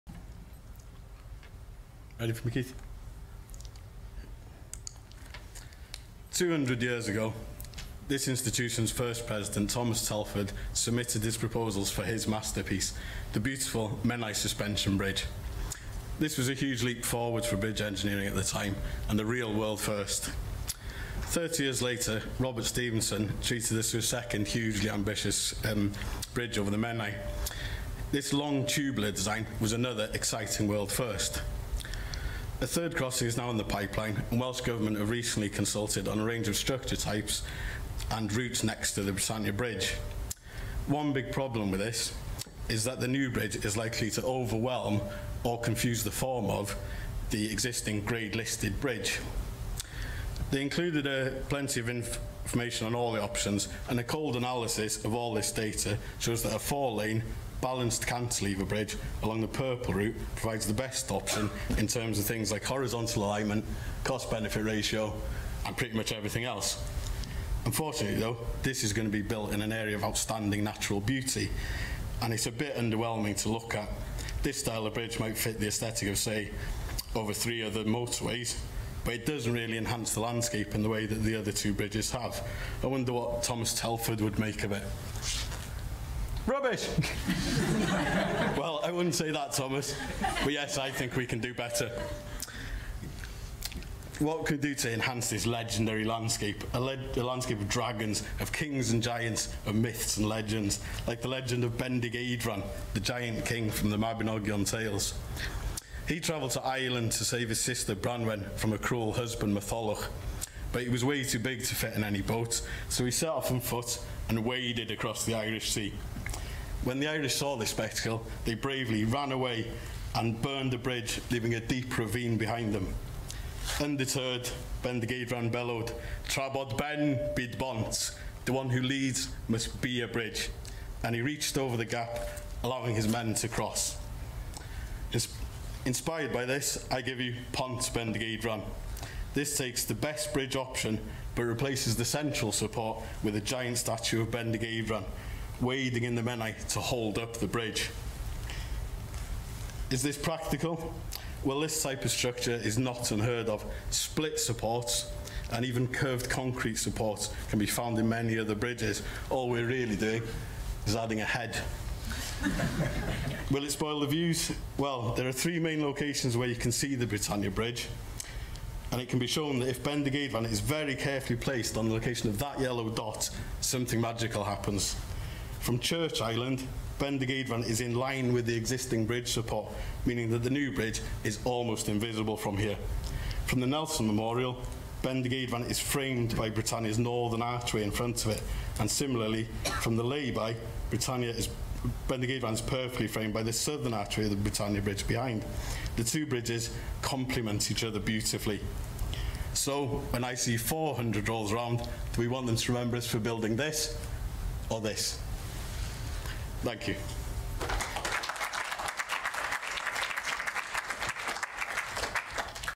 Pont Bendigeidfran
200 second talk about Pont Bendigeidfran. Given at the Welsh final of the Institution of Civil Engineers 'Pitch 200' competition, celebrating 200 years since the institution was founded. Cardiff 26th April 2018.

Cyflwyniad 200 eiliad am Pont Bendigeidfran, o rownd terfynol Cymru o cystadleuaeth 'Pitch 200' yr ICE. Caerdydd, Ebrill 26ed 2018.